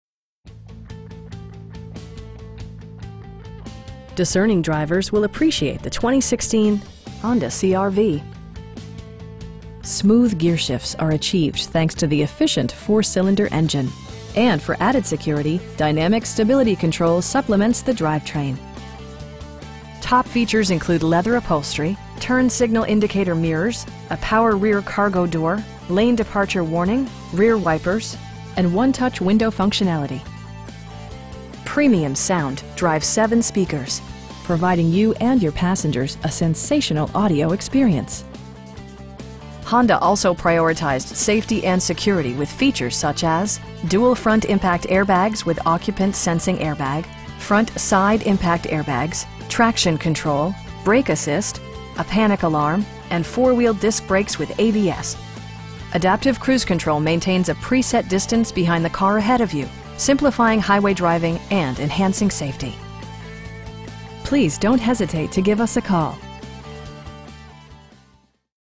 2016 Honda CR-V Touring FWD in Orlando, FL 32837
11051 S Orange Blossom Trail

Discerning drivers will appreciate the 2016 Honda CR-V. Smooth gearshifts are achieved thanks to the efficient 4 cylinder engine, and for added security, dynamic Stability Control supplements the drivetrain. Top features include leather upholstery, air conditioning, turn signal indicator mirrors, a power rear cargo door, lane departure warning, an overhead console, rear wipers, and 1-touch window functionality. Premium sound drives 7 speakers, providing you and your passengers a sensational audio experience. Honda also prioritized safety and security with features such as: dual front impact airbags with occupant sensing airbag, front SIDE impact airbags, traction control, brake assist, a panic alarm, and 4 wheel disc brakes with ABS. Adaptive cruise control maintains a pre set distance behind the car ahead of you, simplifying highway driving and enhancing safety. We pride ourselves in consistently exceeding our customer's expectations. Please don't hesitate to give us a call.